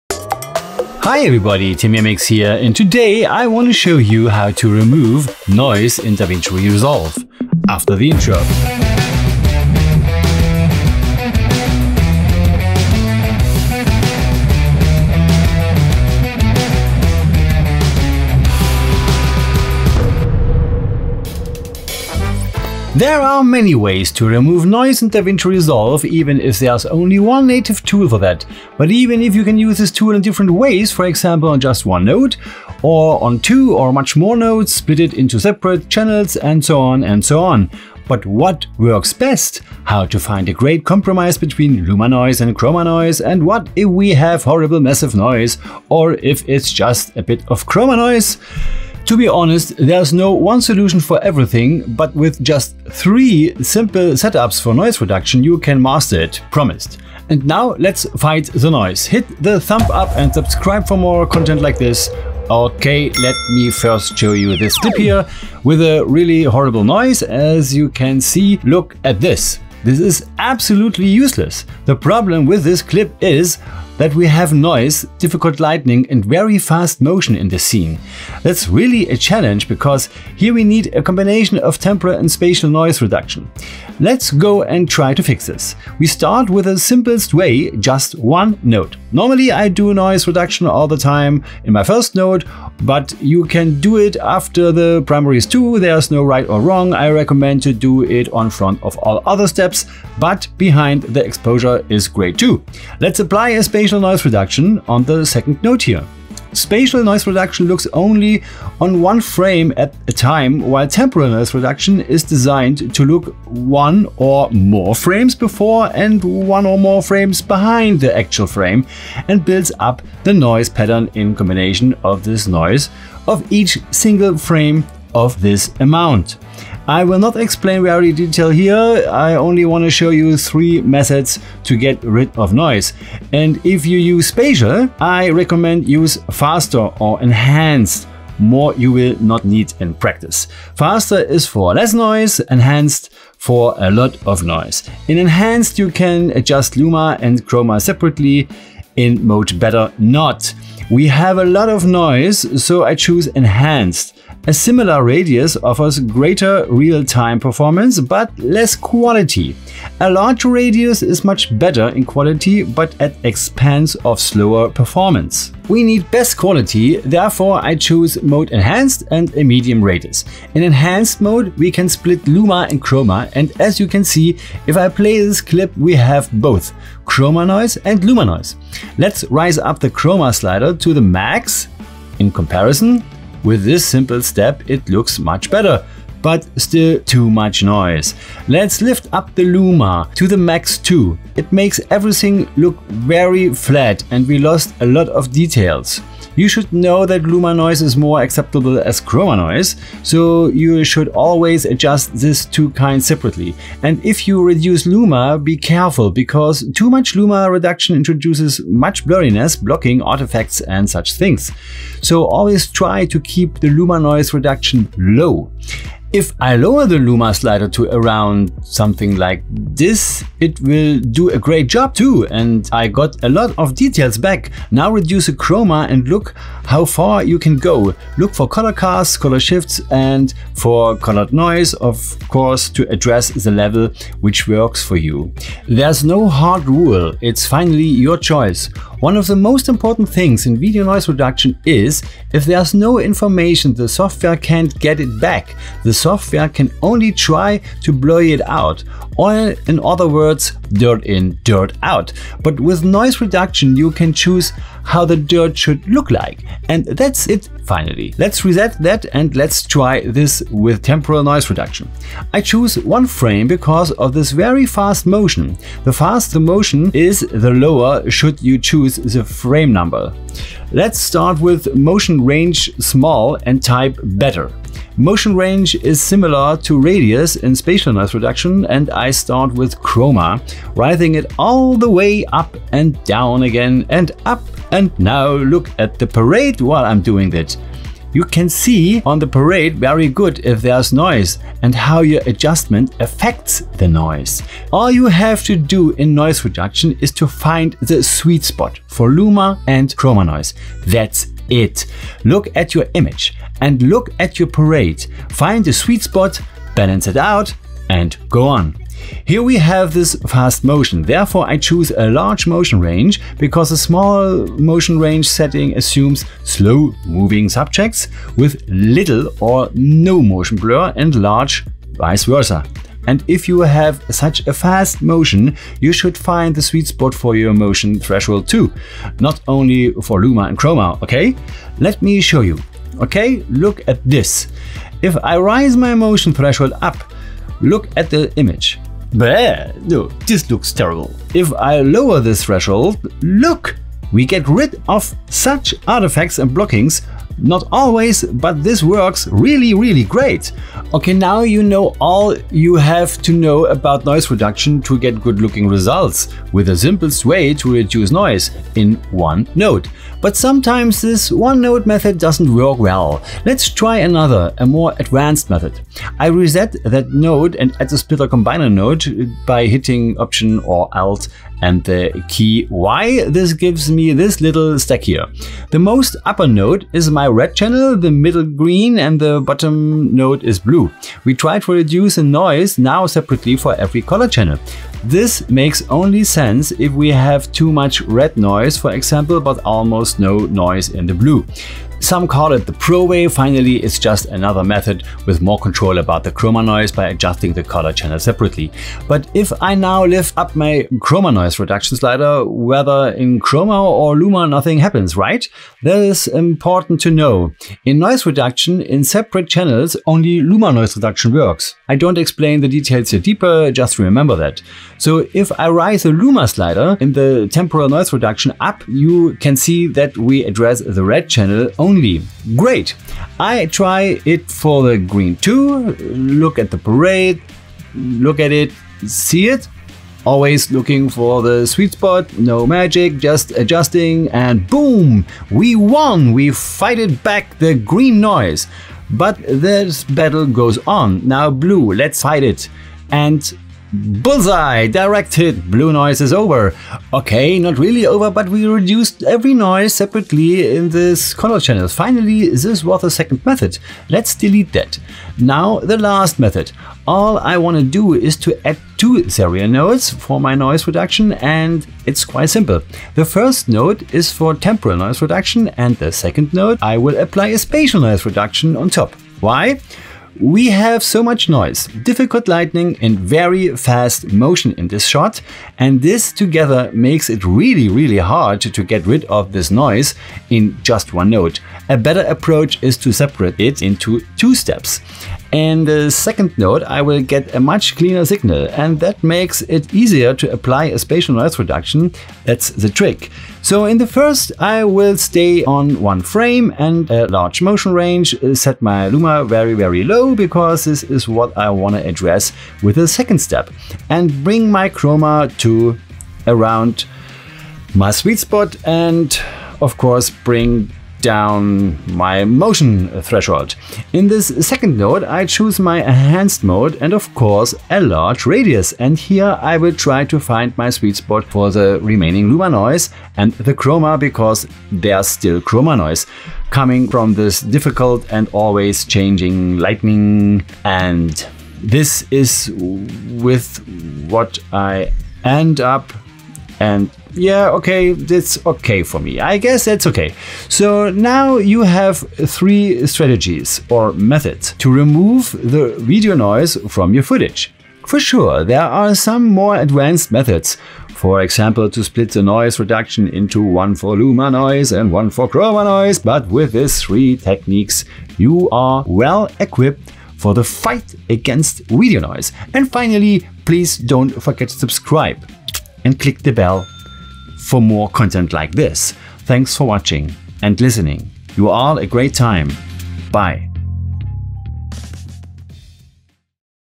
How to Remove Noise - Noise Reduction Tutorial for DaVinci Resolve
In this video noise reduction tutorial for DaVinci Resolve I show you how to remove noise with 3 simple methods. That's all you need to get rid of the video noise, those methods works for the most cases.

No gear or software was supplied to me for this video.